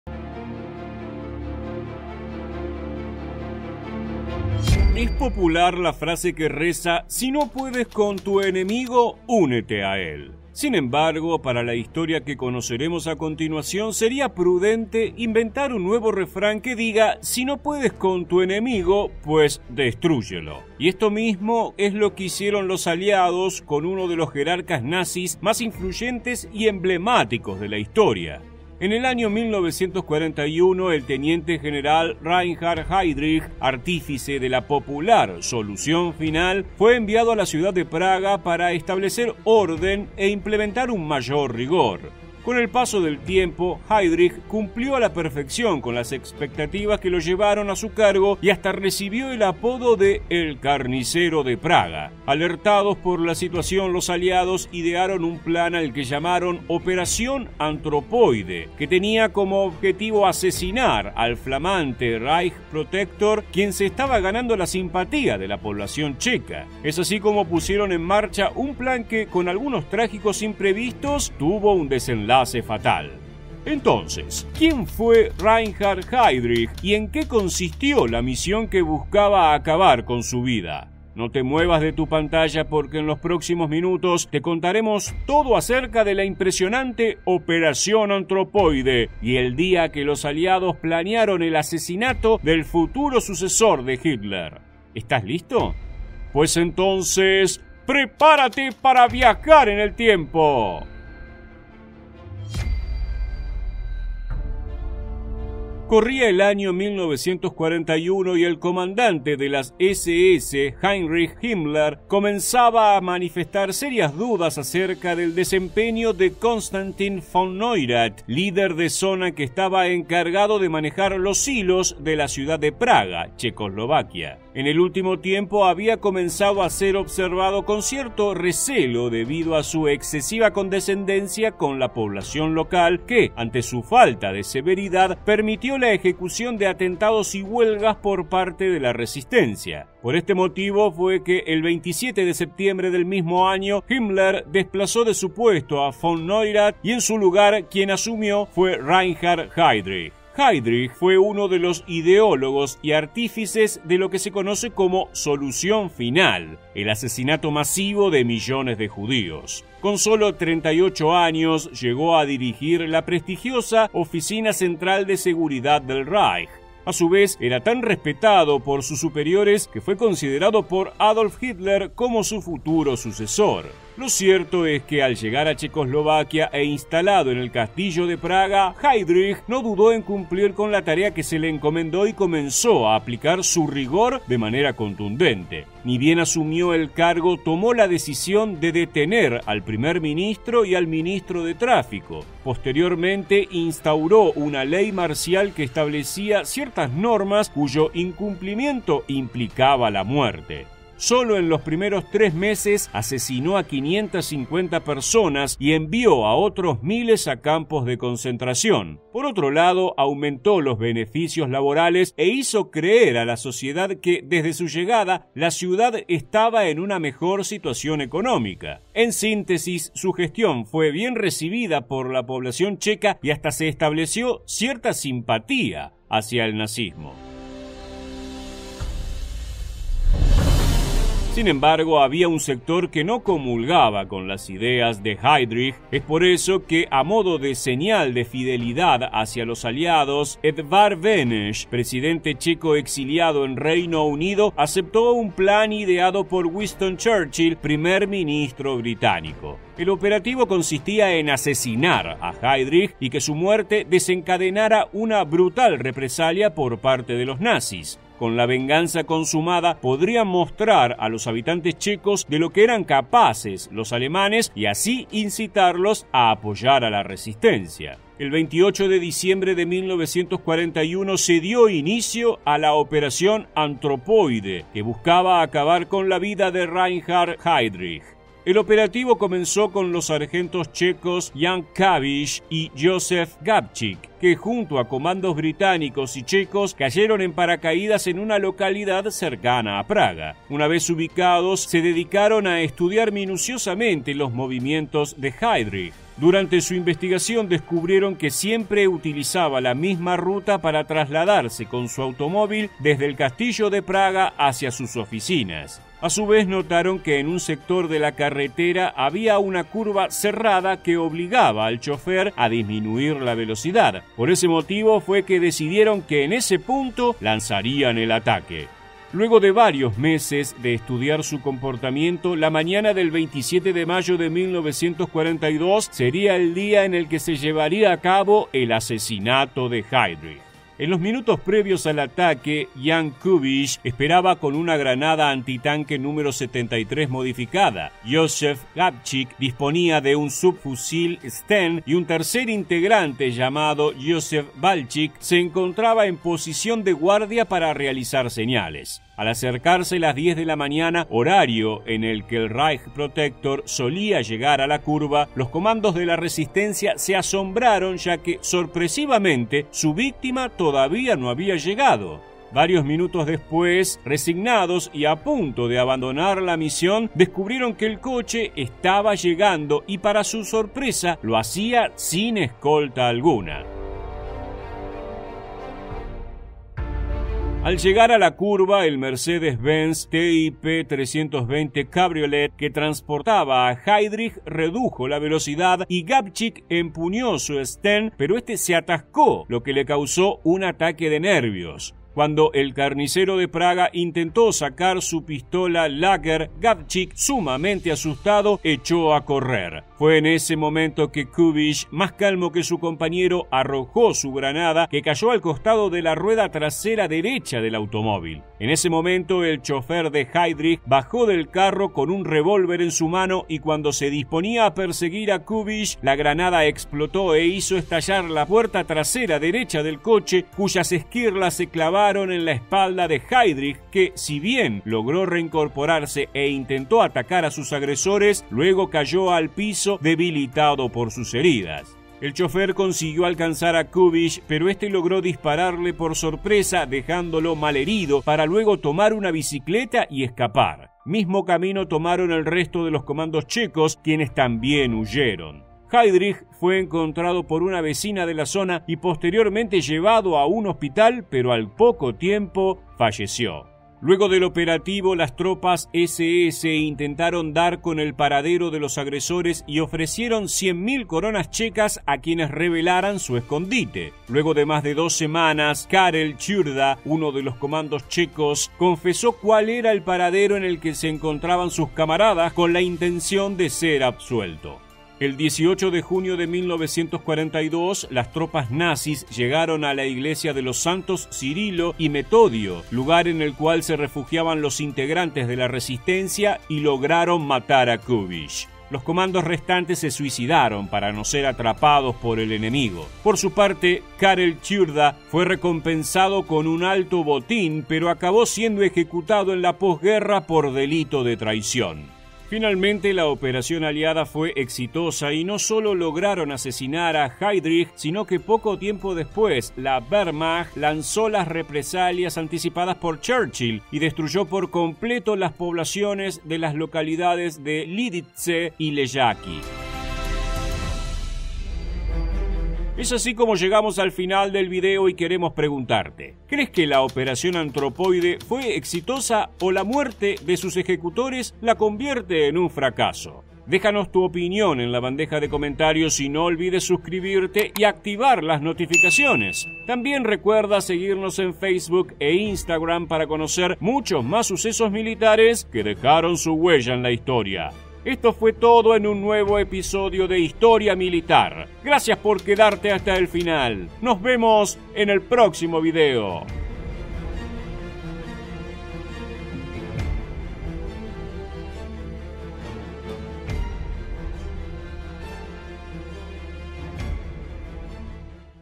El brutal PLAN ALIADO para ANIQUILAR al SUCESOR de Hitler I Operación Antropoide
¿Cómo fue el astuto plan para asesinar al sucesor de Hitler? ¡Si quieres descubrir cómo fue la misión aliada ideada por Churchill para aniquilar a Reinhard Heydrich no te pierdas este video!
En 1941, Reinhard Heydrich, un posible sucesor de Hitler, asumió como Reichsprotektor de Praga para imponer mano dura en la ciudad. Luego de varios meses de hostigamiento, Winston Churchill ideó un plan junto a la resistencia checa para asesinar a Heydrich. Es así como el 27 de mayo de 1942, luego de una gran investigación, un comando de checos y británicos esperó al jerarca en la ruta para realizarle una emboscada.

¿Quieres conocer otras misiones aliadas de la Segunda Guerra Mundial? ¡Aquí te dejo! 👇